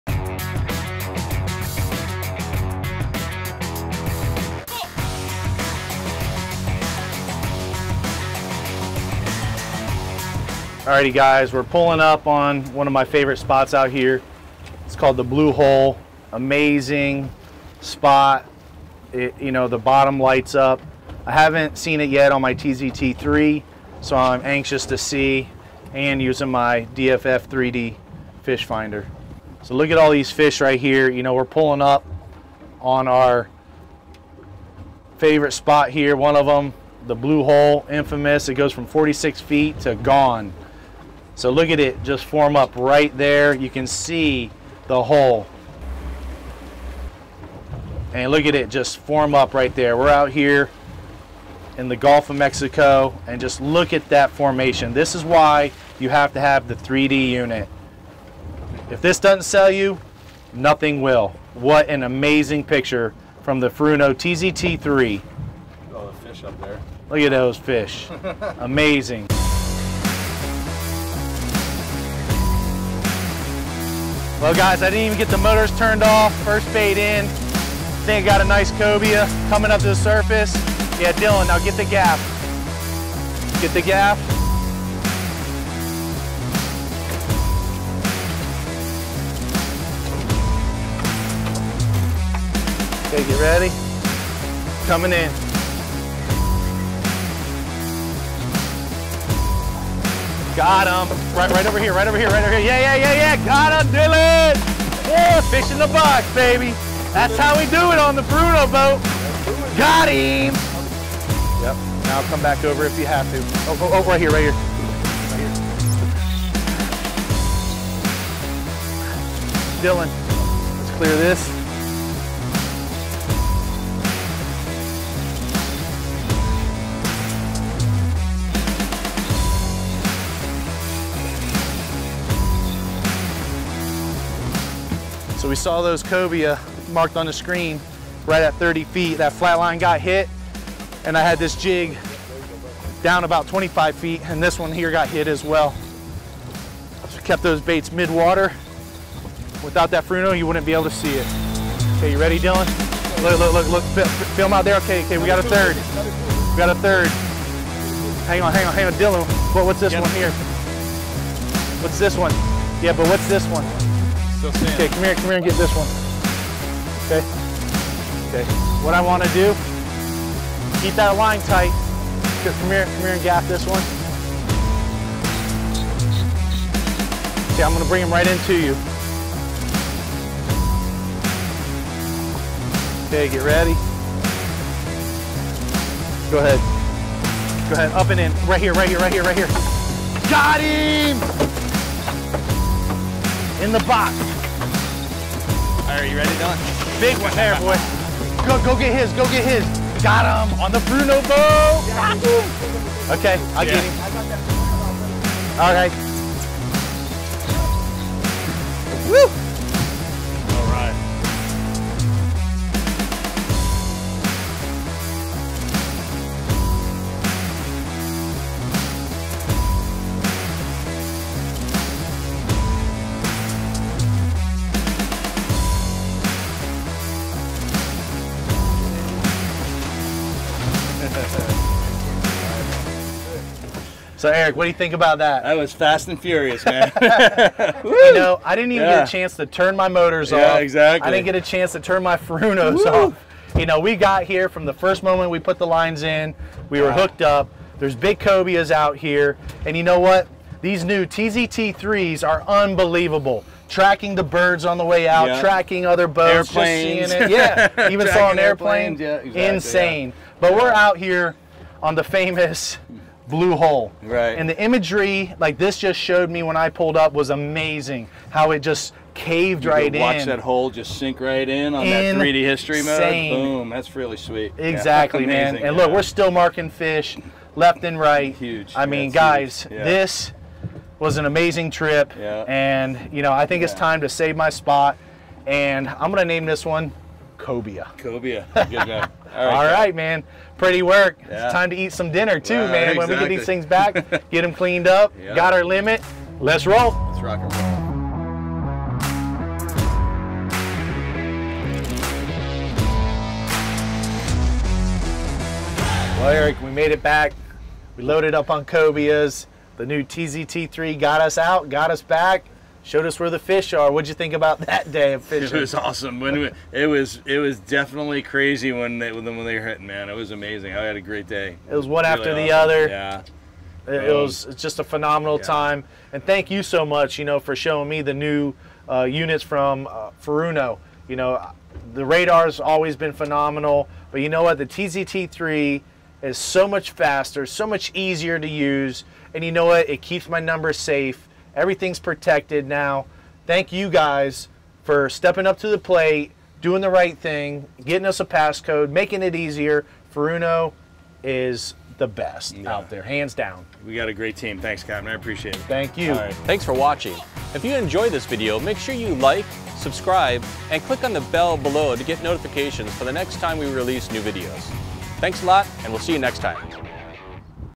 Furuno Connections - Quick Take - Fishing for Cobias in the Blue Hole
Capt. Jack Carlson takes us out fishing to the Blue Hole in the Gulf of Mexico, where we put Furuno's TZtouch3 internal 1kW TruEcho CHIRP Fish Finder and the DFF3D Multi Beam Sonar to the test. As we come upon the Blue Hole, the DFF3D draws a spectacular picture of the bottom and the fish feeding on the bait. Before we can turn off the engines, the Cobia bite is on and the adventure takes off.

Starring
Capt. Jack Carlson – Two Conchs Sportfishing
Eric Kunz – Furuno Senior Product Manager